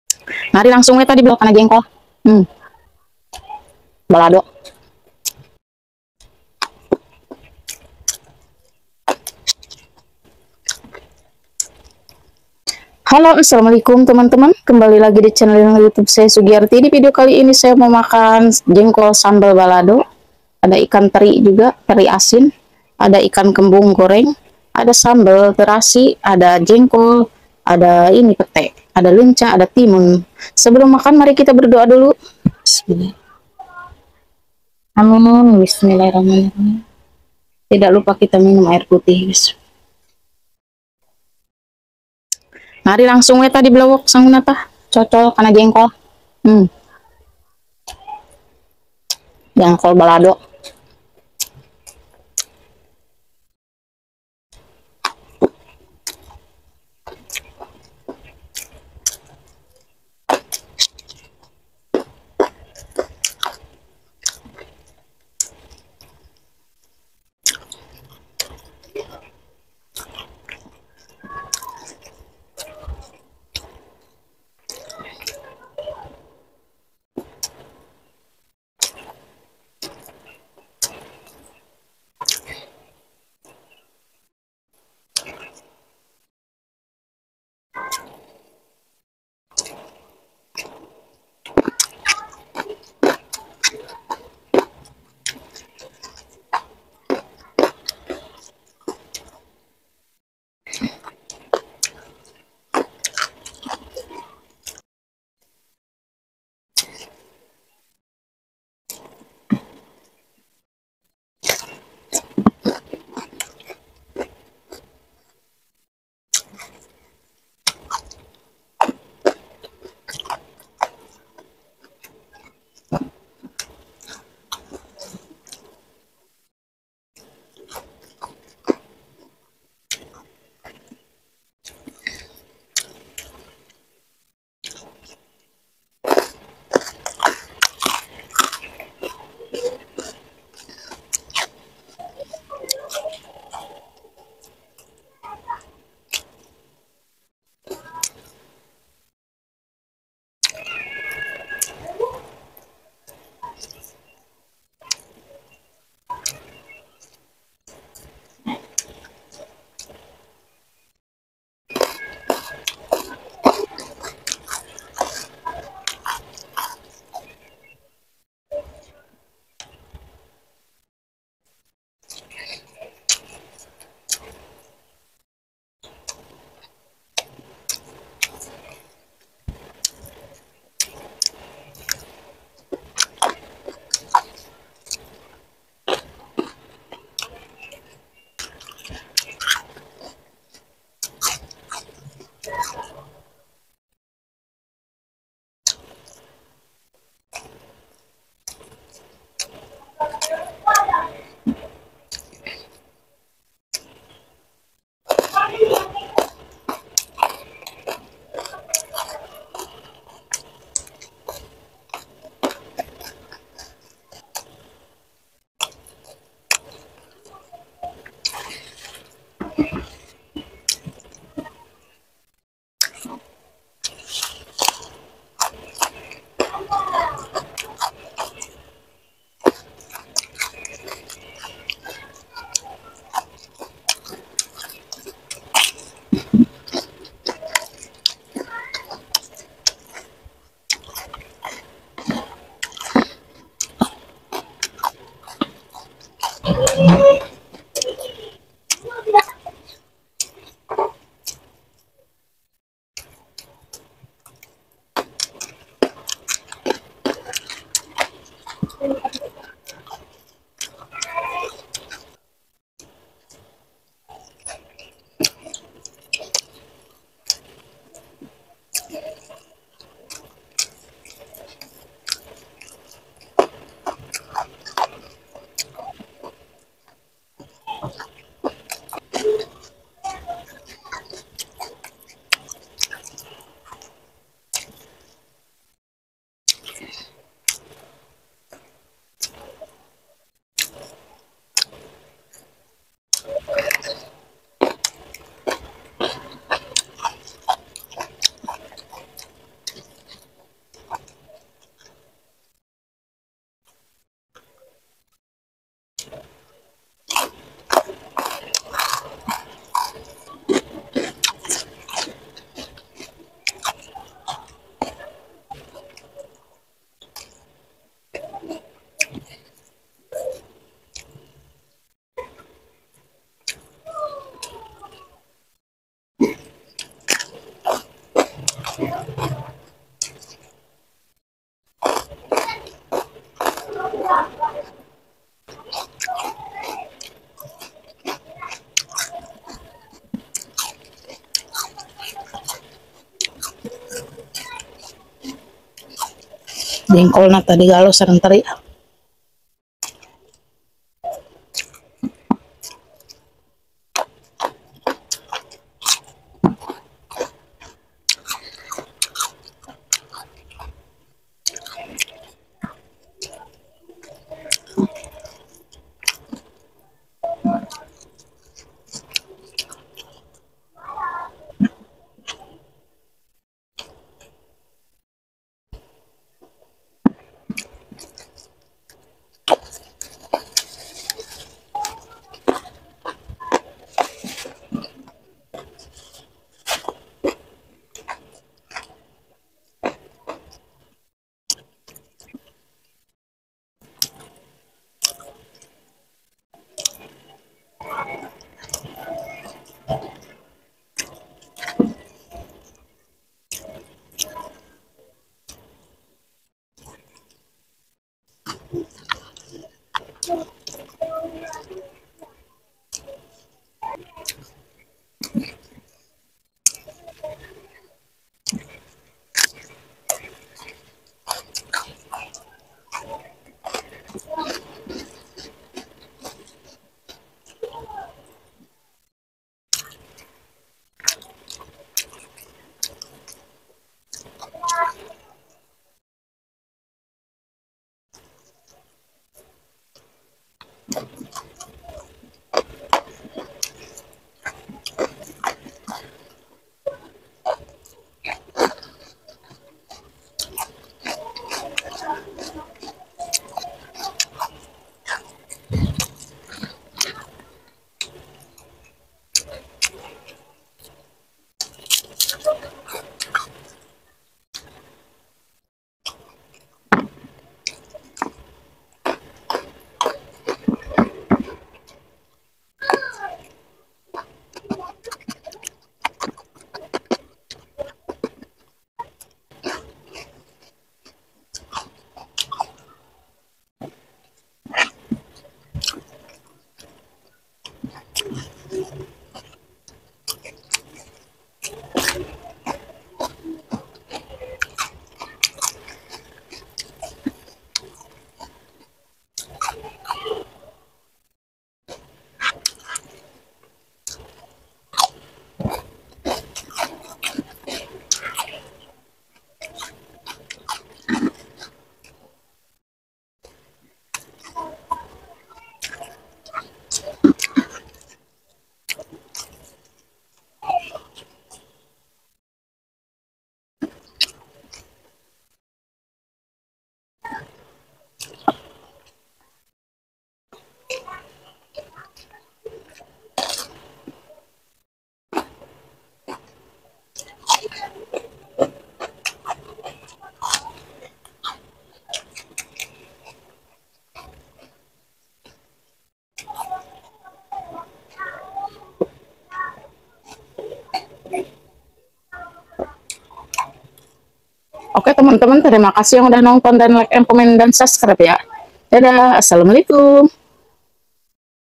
MAKAN JENGKOL BALADO PEDAS ‼️ IKAN KEMBUNG GORENG SAMBEL TERASI LALAPAN JENGKOL MENTAH PETE LEUNCA
assalamualaikum teman2 video kali ini makan sederhana jengkol balado pedas dan lalapan ikan kambung mukbang youtube lalapanmentah @sugiarti2293